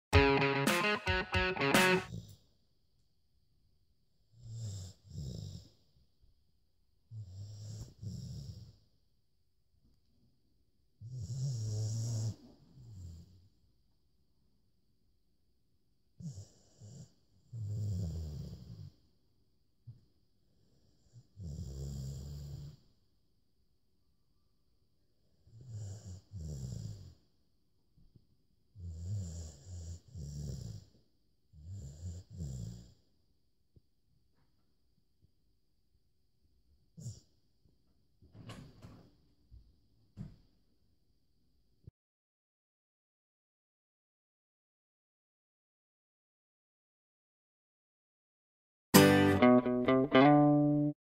TIMMAY Sleeping
Ned's family dog TIMMAY sleeps...oddly.

He awakens...definitively...